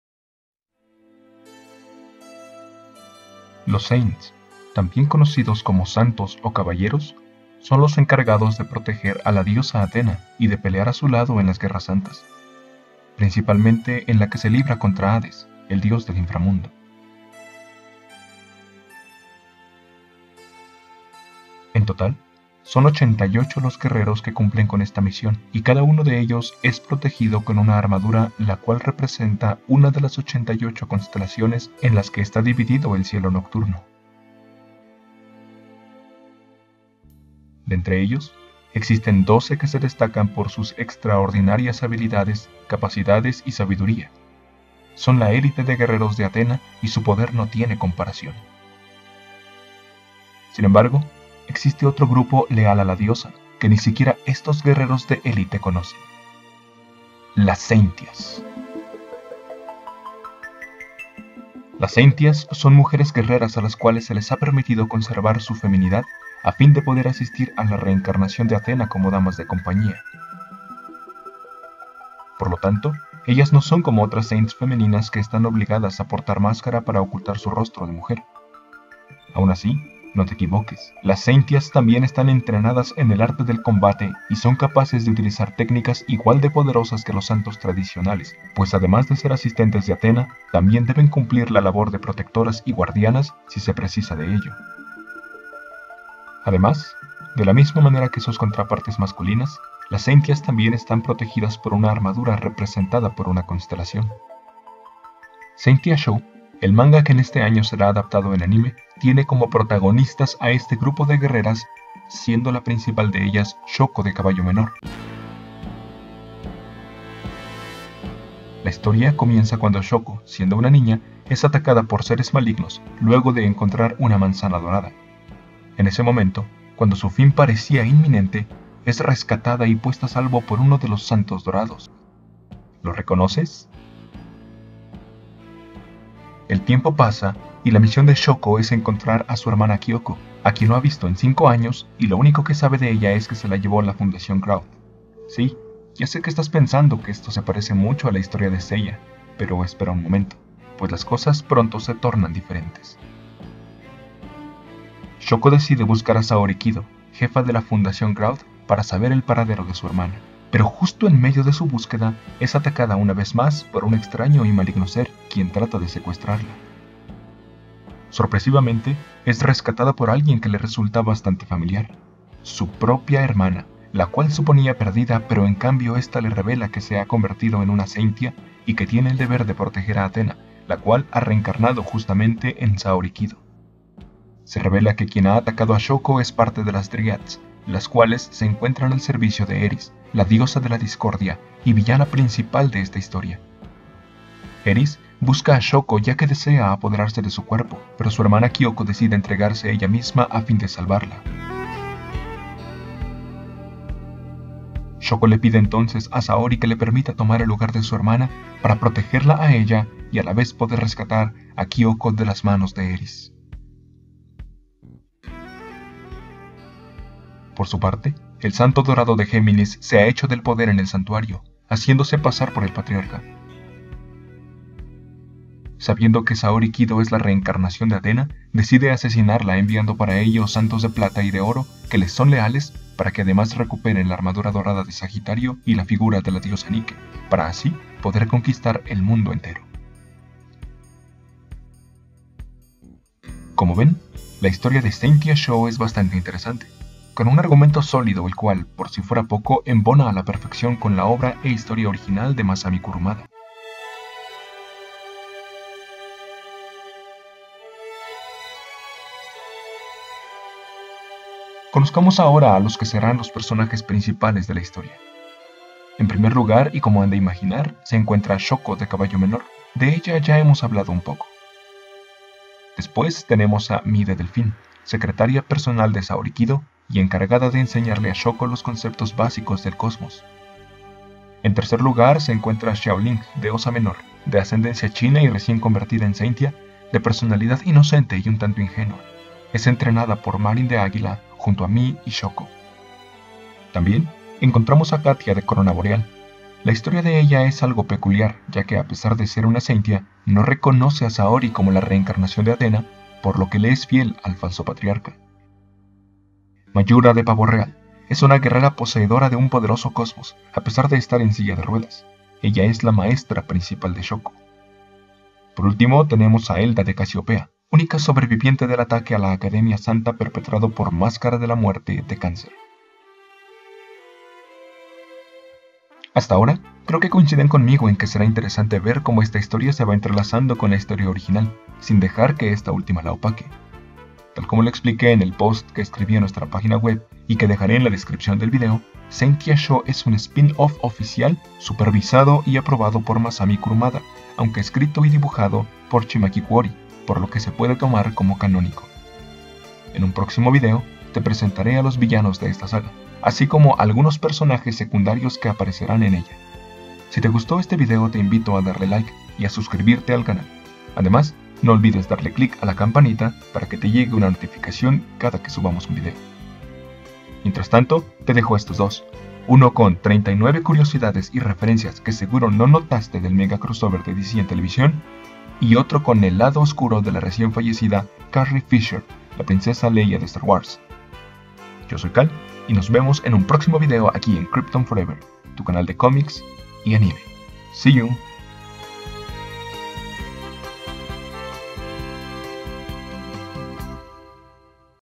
SAINTIA SHO | ¿De qué tratará el NUEVO ANIME de Saint Seiya?
Este 2017 tendremos el nuevo anime de Saint Seiya (Caballeros del Zodiaco). ¿De qué tratará? ¿Es recomendable verlo?